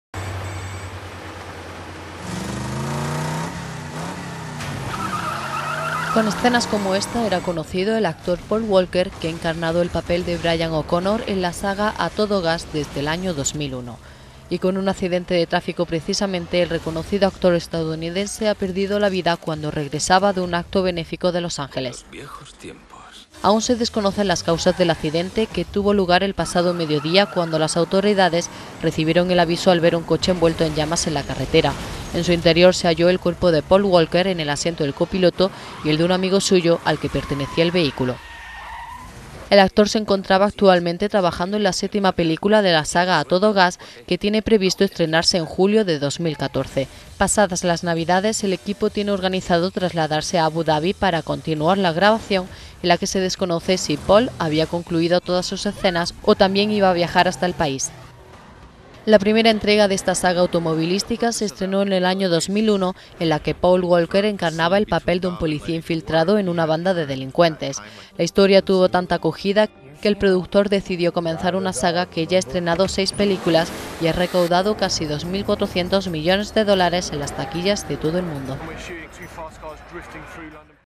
Muere el actor Paul Walker en un accidente de tráfico
Aún se desconocen las causas del accidente, que tuvo lugar pasado el medio día, cuando las autoridades recibieron el aviso al ver un coche envuelto en llamas en la carretera. En su interior se halló el cuerpo Paul Walker, en el asiento del copiloto, y el de un amigo suyo, al que pertenecía el vehículo.